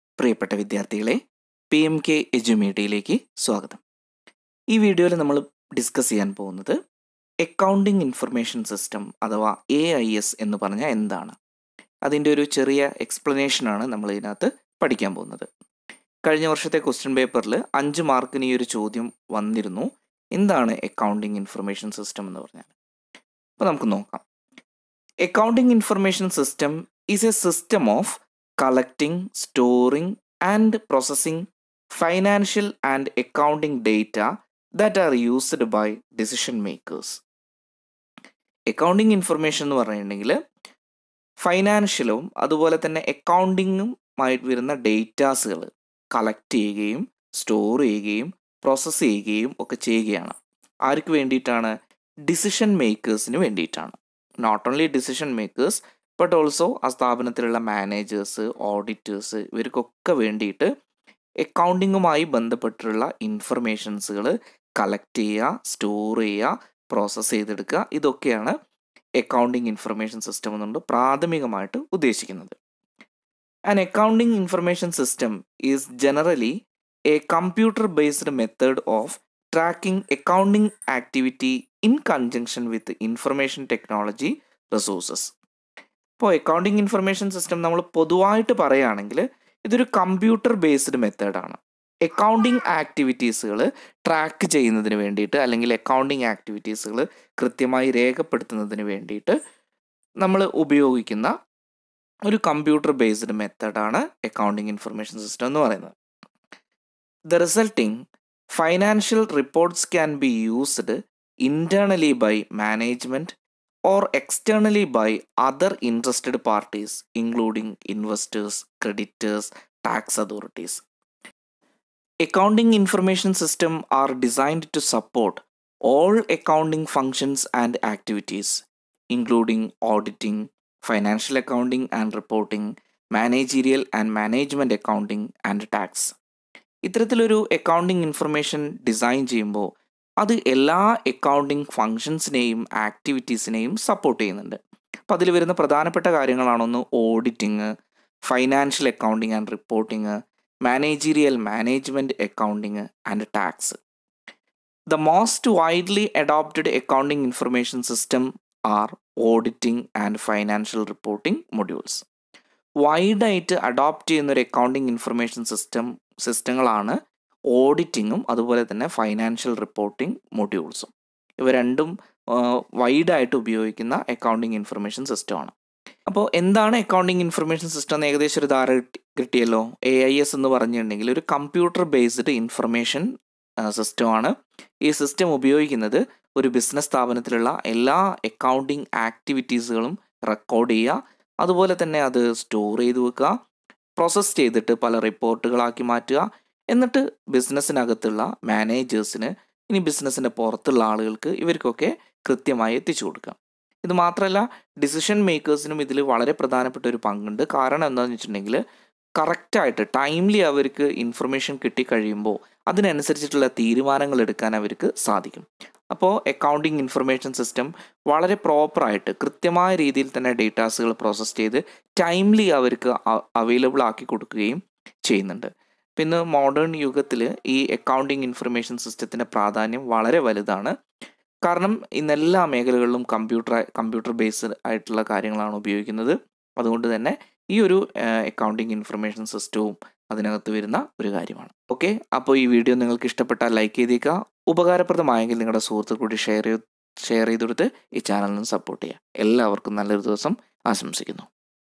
Accounting Information System (AIS) | MALAYALAM | Financial Accounting
Content : This video explains Accounting Information System.